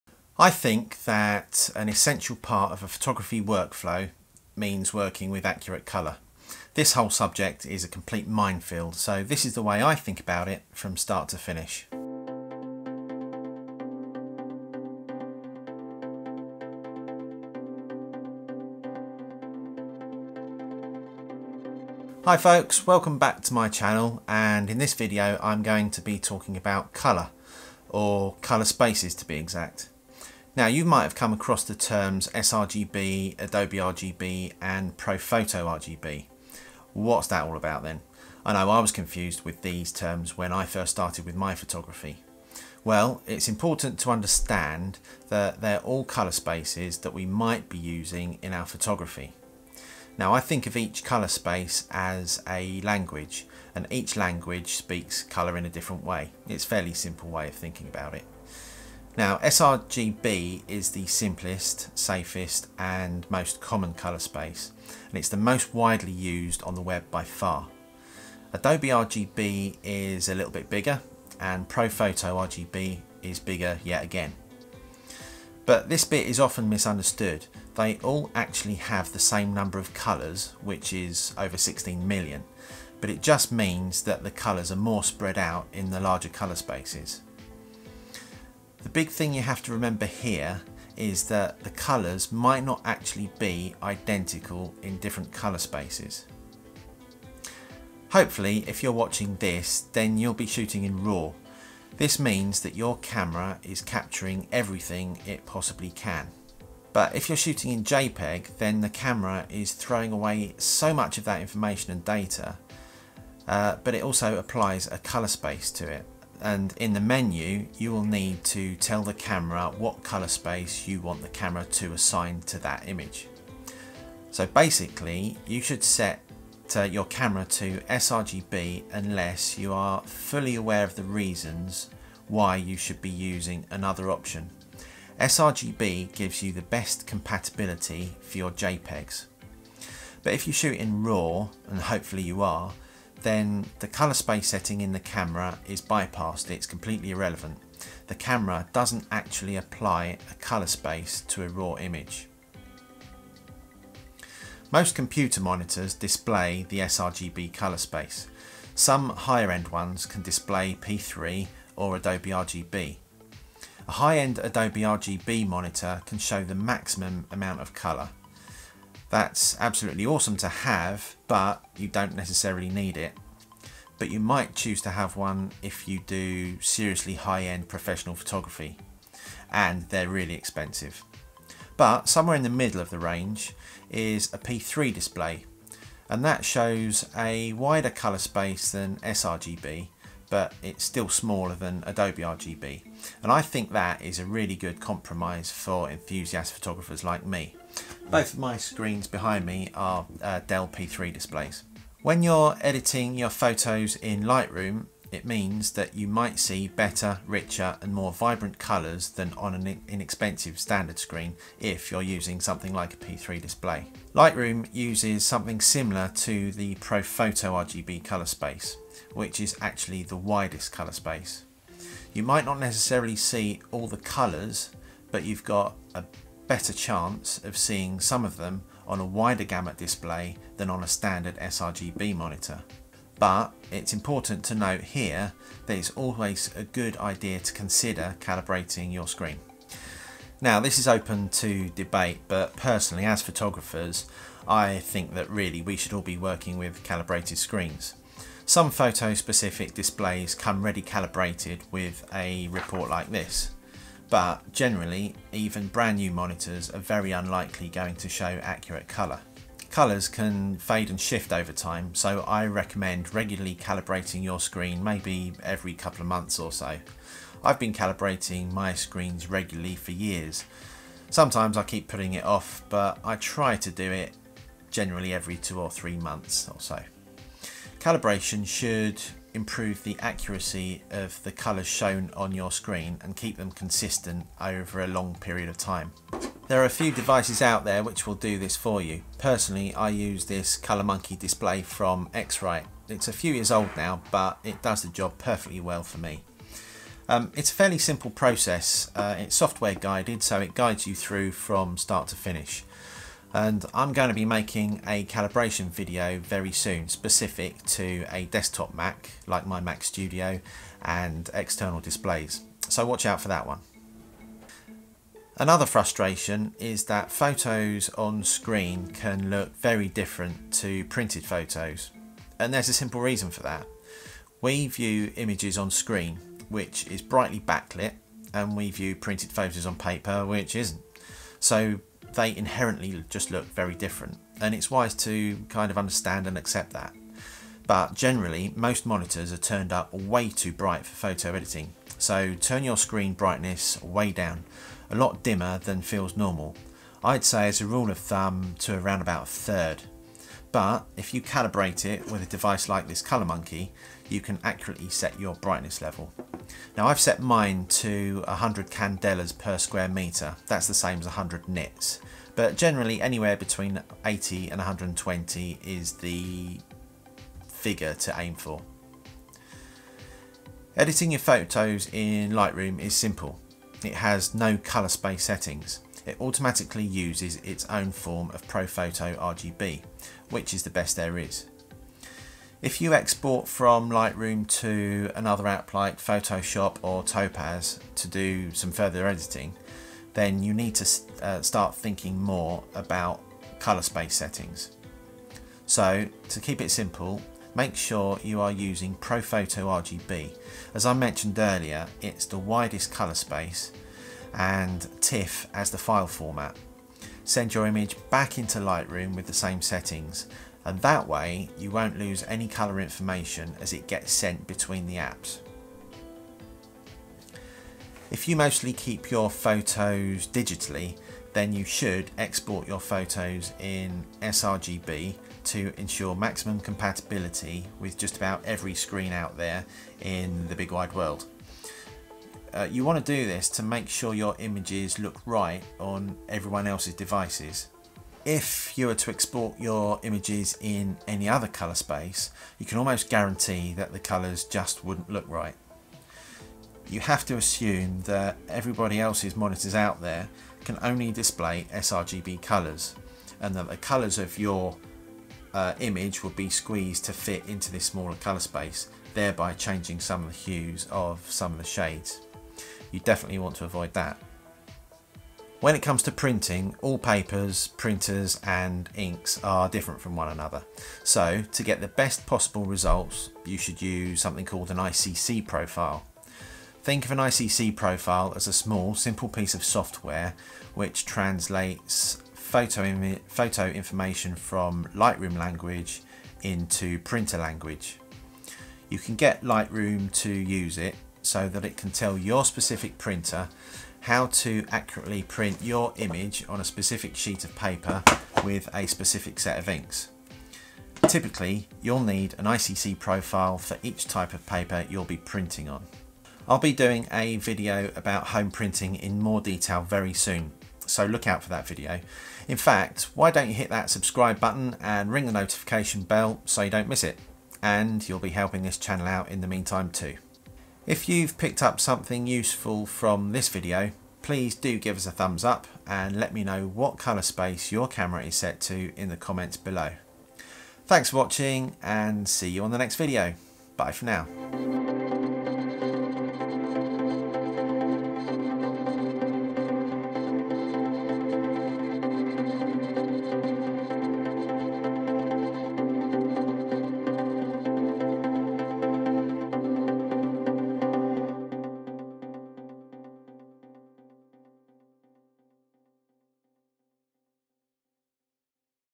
Colour Workflow and Colour Spaces Explained | Photography and Photo Editing
My colour workflow and colour spaces are explained in this slightly tech-based video - do you know about the differences between sRGB, Adobe RGB, Pro Photo RGB and DCI-P3?

I look at the basics of colour management for a good and accurate photography workflow in this video and talk through the way I deal with colour spaces and colour settings from shoot to edit to print.

Stu.

📷 My photography gear 👇
Canon Cameras and Lenses
Shimoda and Lowe Pro Camera Bags
Benro Tripod
Lee 100 Filters